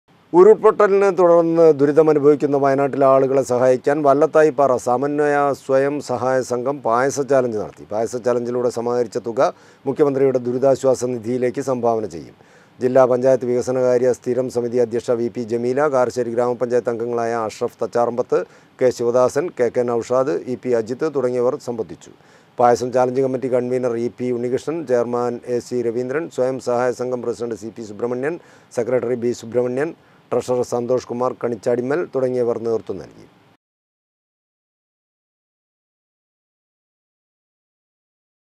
വയനാട്ടിലെ ആളുകളെ സഹായിക്കാൻ വല്ലത്തായിപ്പാറ സമന്വയ സ്വയം സഹായ സംഘം പായസ ചലഞ്ച് നടത്തി.
ഉരുൾപൊട്ടലിനെ തുടർന്ന് ദുരിതമനുഭവിക്കുന്ന വയനാട്ടിലെ ആളുകളെ സഹായിക്കാൻ വല്ലത്തായിപ്പാറ സമന്വയ സ്വയം സഹായ സംഘം പായസ ചലഞ്ച് നടത്തി.